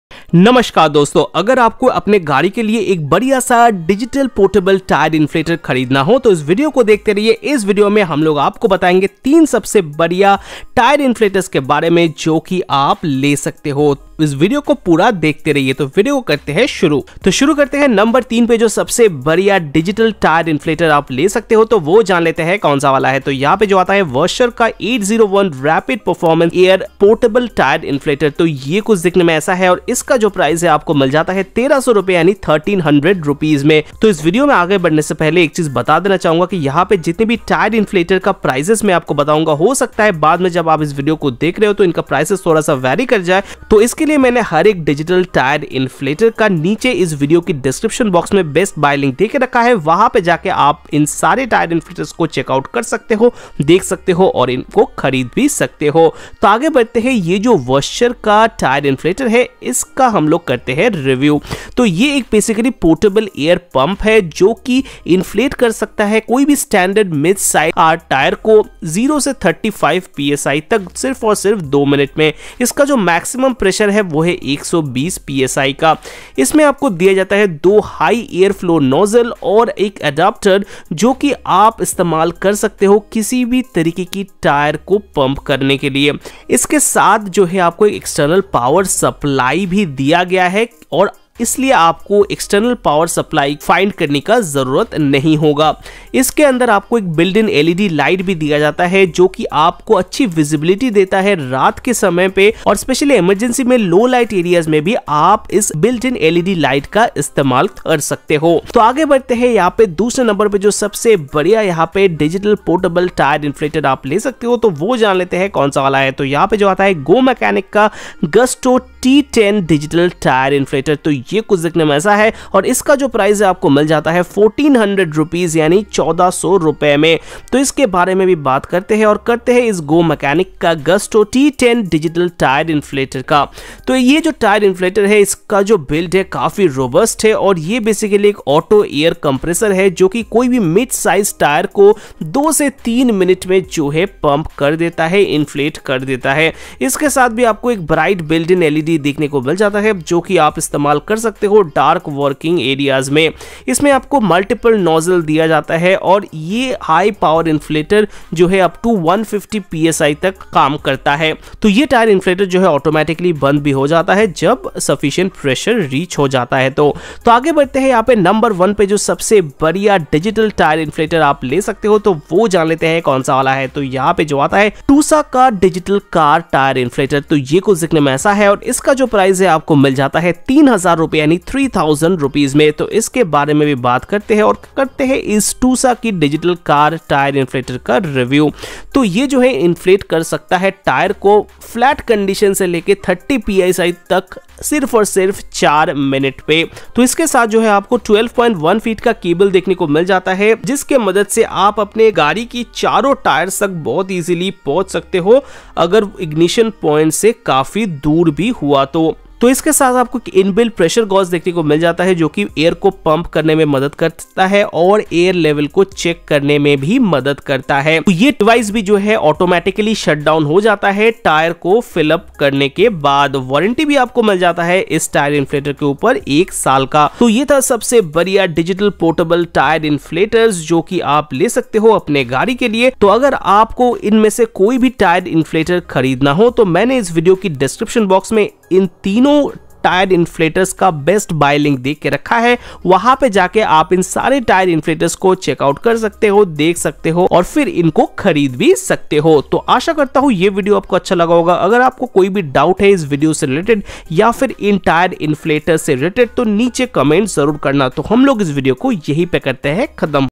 Best 3 Tyre Inflator in India 2024
In this video we have done review of the Top 3 Best Portable Digital Tyre Inflators that  you can use for your car or bike.

1. TUSA Digital Car Tyre Inflator

2. GoMechanic Gusto T10 Digital Tyre Inflator

3. Woscher 801 Rapid Performance Car Air Pump Portable Tyre Inflator